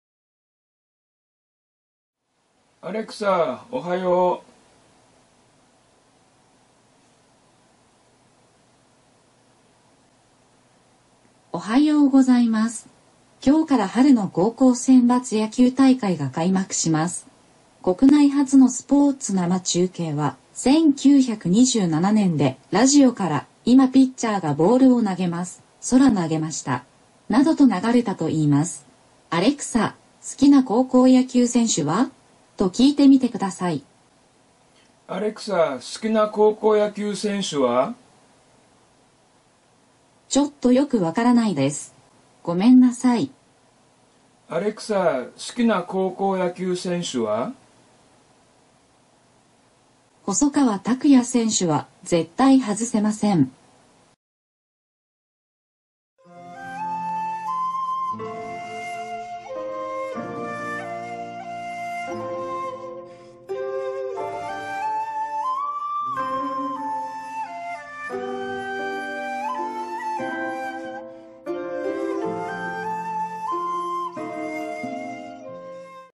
【Amazon Echo今日のこよみ】アレクサ、春のセンバツを語る。
今日から春の高校選抜野球大会が開幕します。国内初のスポーツ生中継は、1927年でラジオから、「いまピッチャーがボールを投げます。ソラ投げました！」などと流れたと言います。「アレクサ、好きな高校選手は？」と聞いてみてください。

Q:アレクサ、好きな高校野球選手は？
A:細川拓哉選手は絶対外せません。

【注】
細川拓哉選手は、明秀学園日立高等学校野球部で、ポジションは投手と三塁手です。お兄さんは、横浜DeNAに入団した細川成也選手です。アレクサの「今日のこよみ」の原稿を作っている担当者は「絶対外せない」というほどですので、細川拓哉選手に注目しましょう。